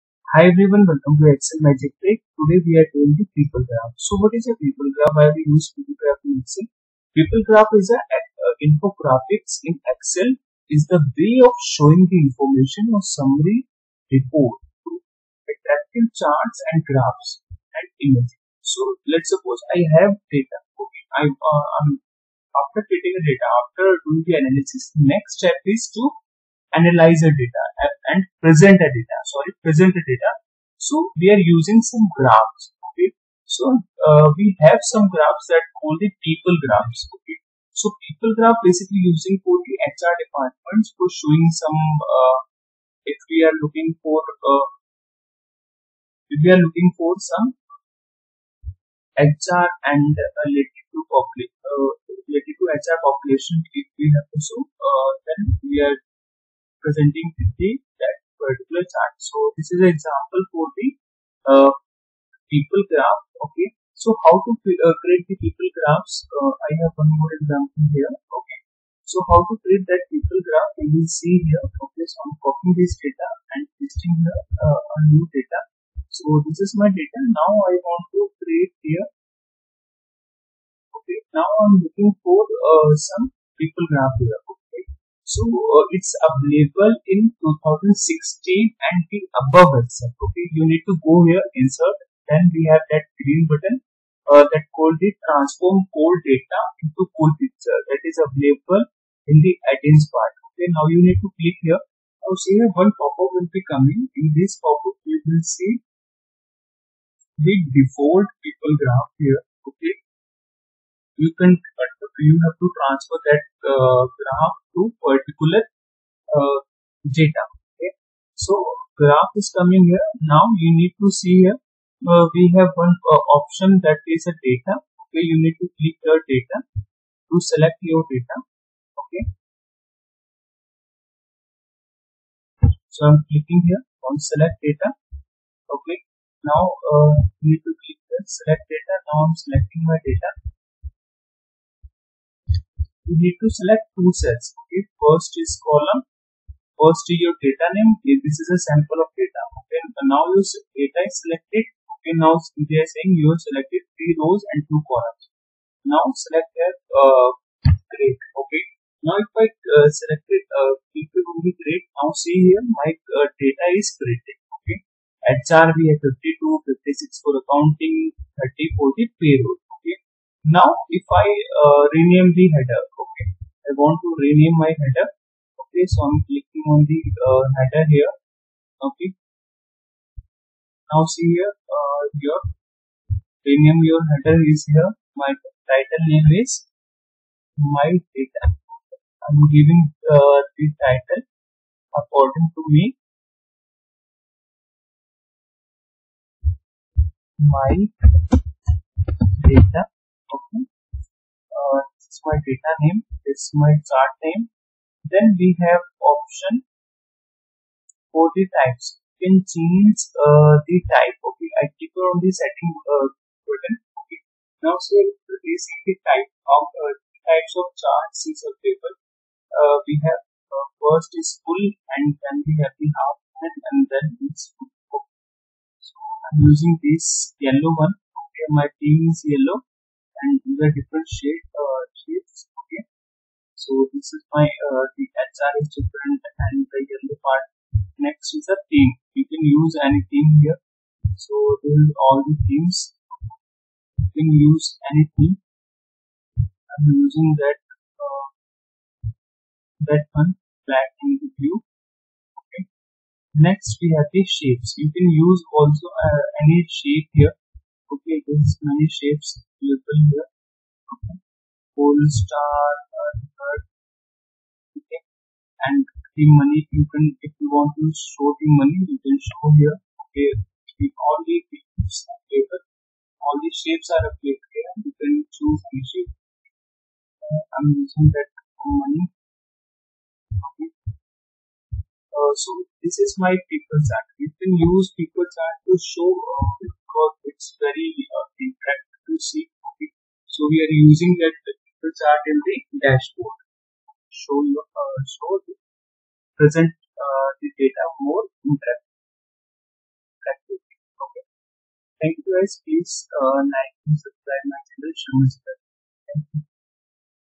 How to Create People Graph or Infographics Charts in Excel (Free Addins)
Infographics in excel is the way of showing the information or summary report through attractive charts, graphs, and images